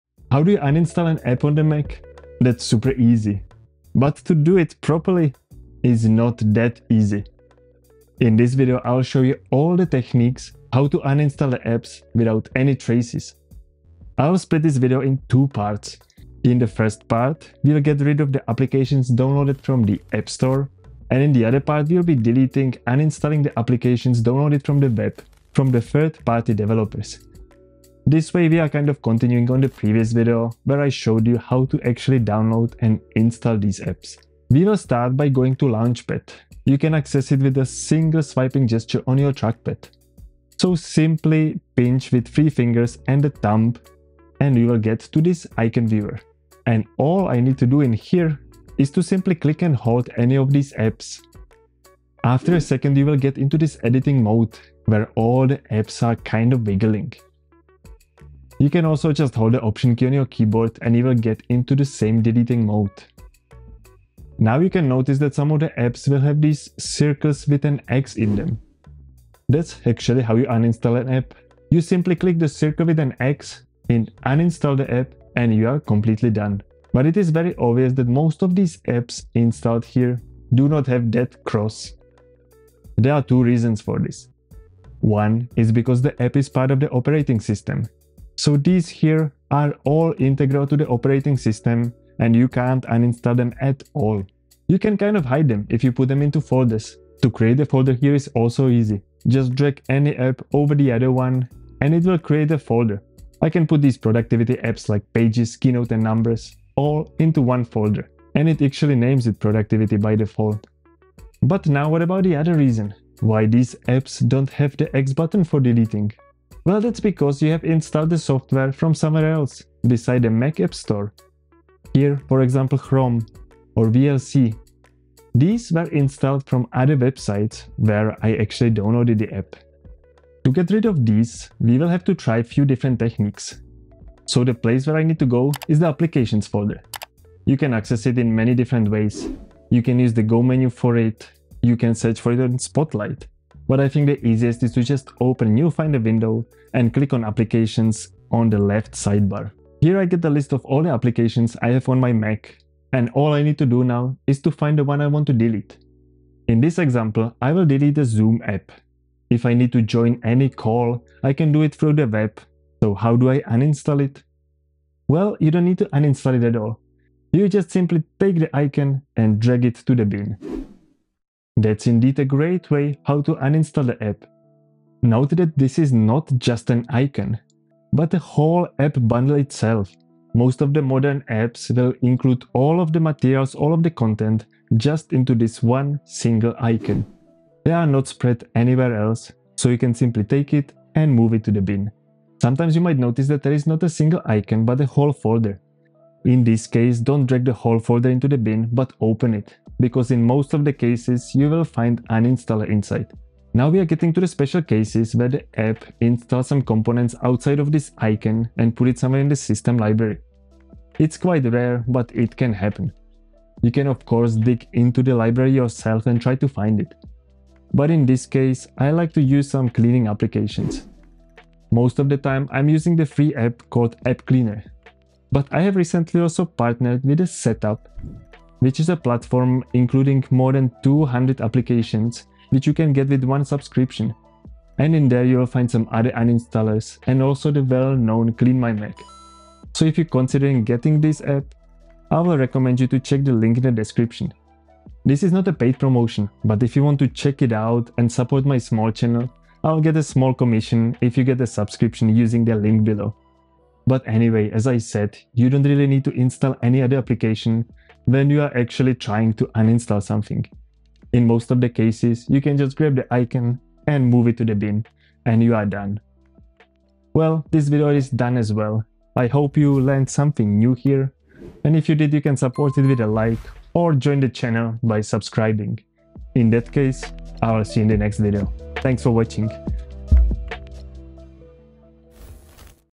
Uninstall Mac Apps the Right Way
How do you uninstall an app on the mac? I will show all the techniques how to uninstall apps without leaving any traces.

Do you want to get more than 200 apps for Mac on one place? Check out Setapp!

⏱ TIMESTAMPS
0:00 - Uninstall apps on Mac
0:34 - Uninstall apps from the AppStore
2:10 - Uninstall 3rd Party Applications
3:23 - Clean all the App traces?